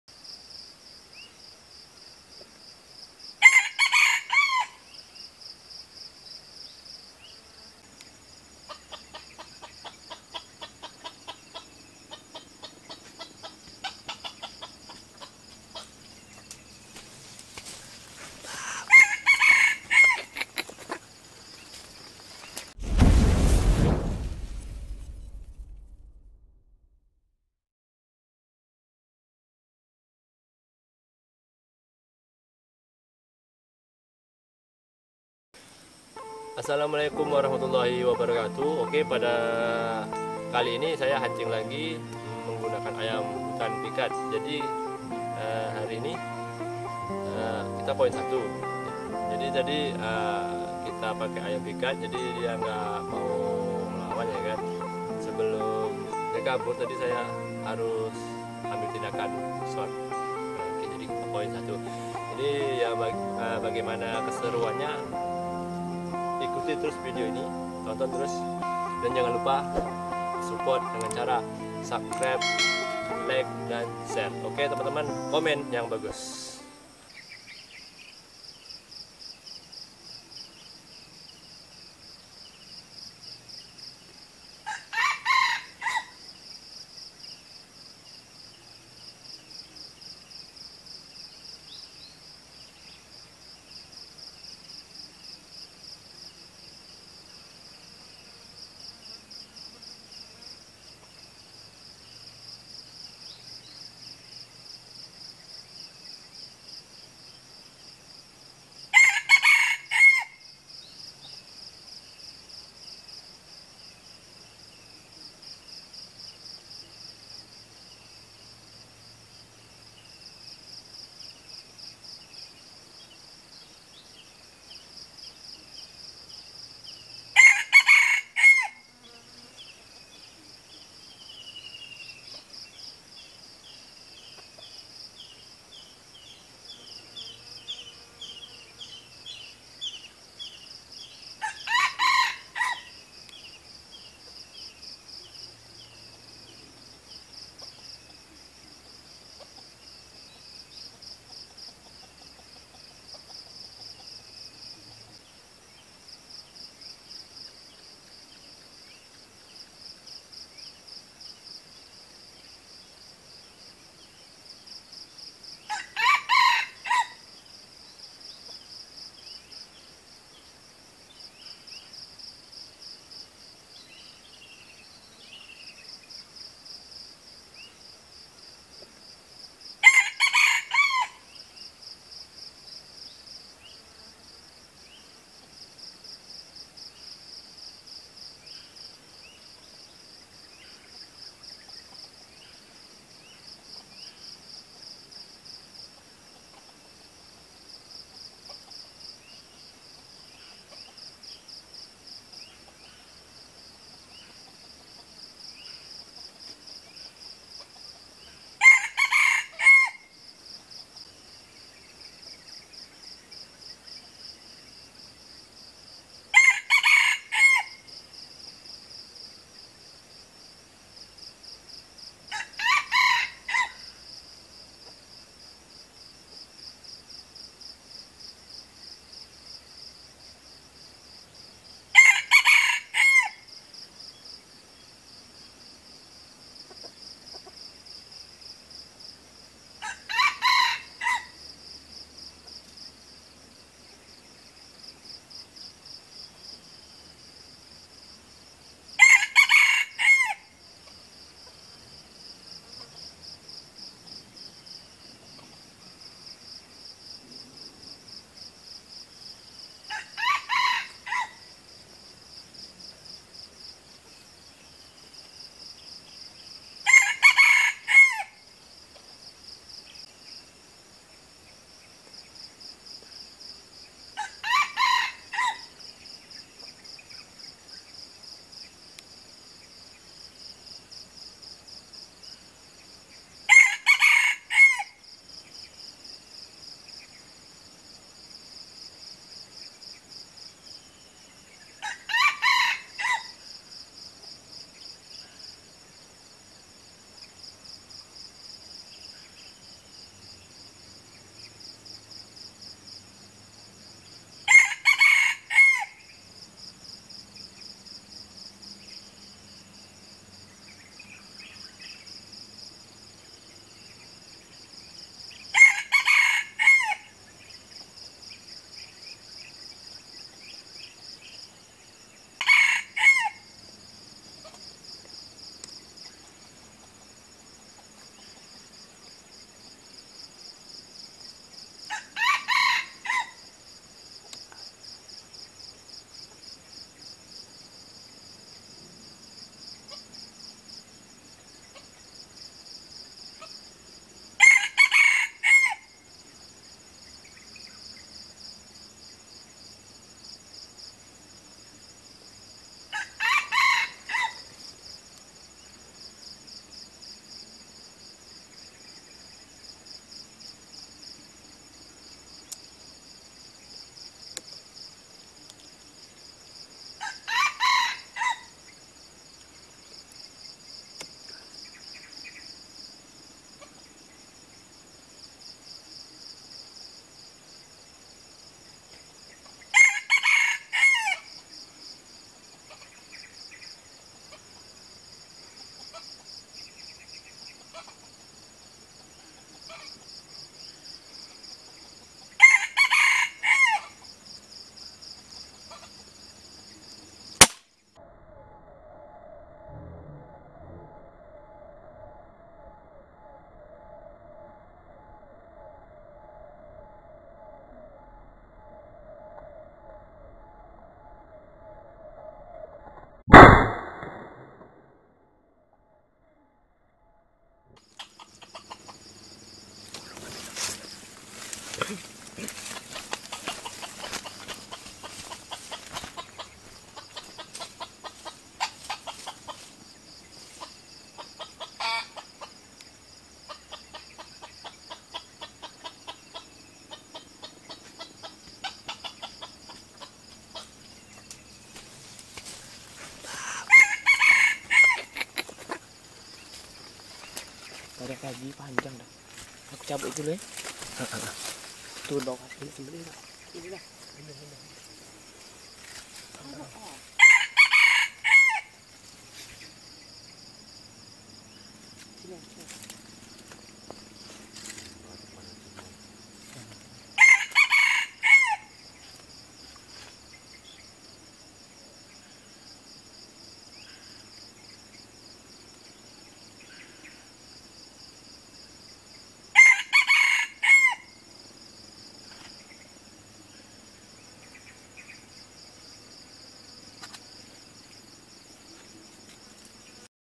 Pikat ayam hutan part 2 | spot sekandang
apa kabar sahabat semua, kali ini kita hunting lagi menggunakan ayam pikat,
terima kasih